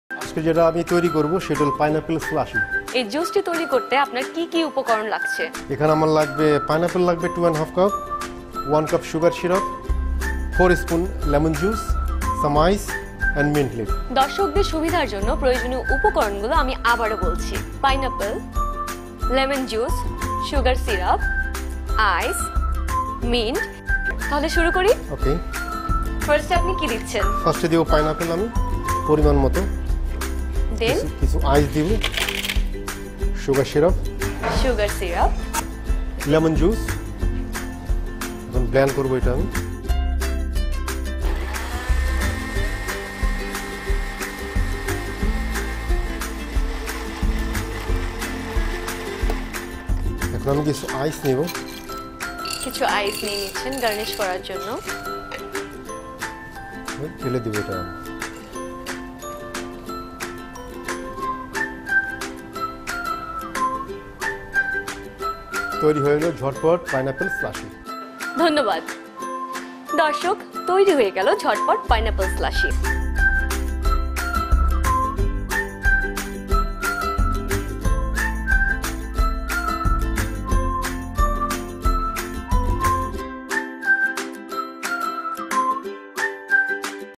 কিভাবে আনারসের স্লেশি বানাবেন | Pineapple Slushies
How to Makw Pineapple Slushies
কিভাবে আনারসের স্লেশি বানাবেন

উপকরন

০১/ আনারস
০২/ সুগার সিরাপ
০৩/ লেমন জুস
০৪/ বরফ কুচি
০৫/ পুদিনা পাতা

প্রস্তুতপ্রণালী

প্রথমে ব্লেন্ডারে এক এক করে সব উপকরন পরিমাণ মত দিয়ে দিন তার পর ভাল করে ব্লেড করুন এর পর সুন্দর করে পরিবেশন করুন  আনারসের স্লেশি।